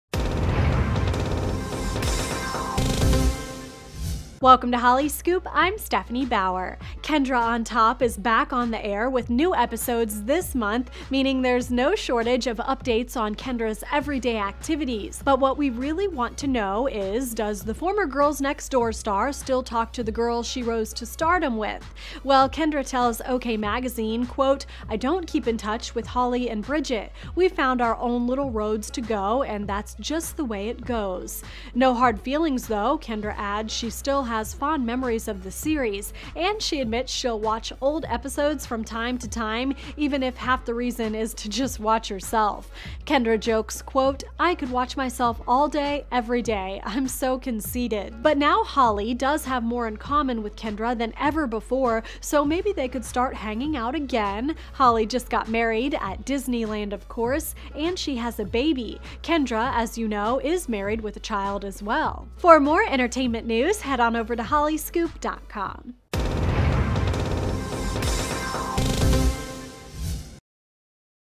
Kendra Wilkinson: Why I Don't Talk to Holly Madison Anymore
"Kendra on Top" is back on the air with new episodes this month, meaning there's no shortage of updates on Kendra Wilkinson's everyday activities.